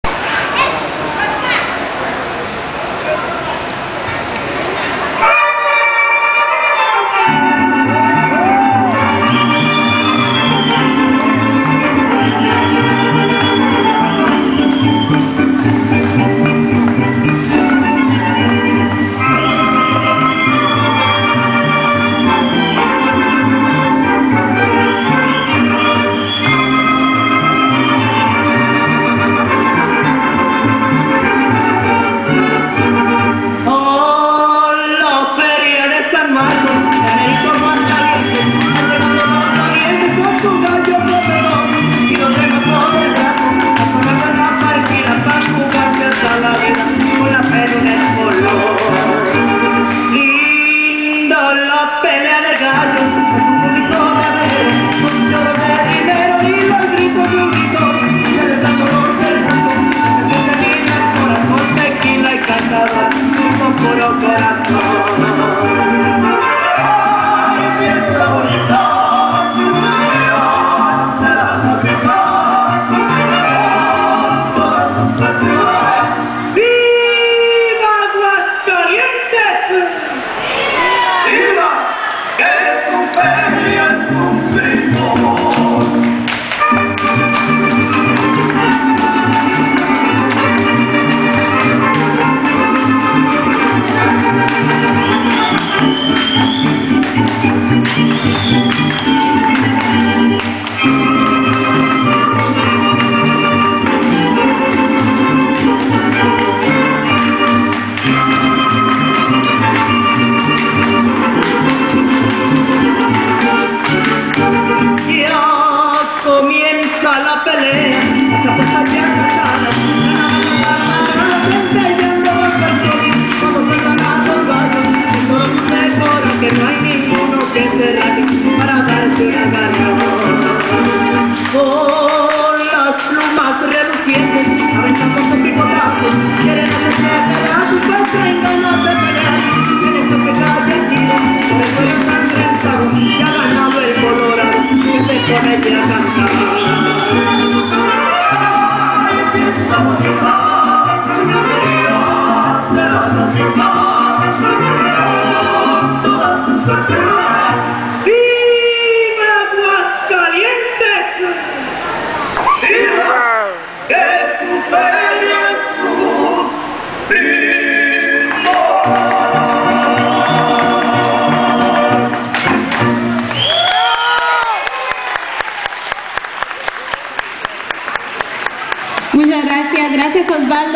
Viva Aguascalientes!!
Folk dance from Mexico: Fiesta de San Marcos at the Fiesta del Pueblo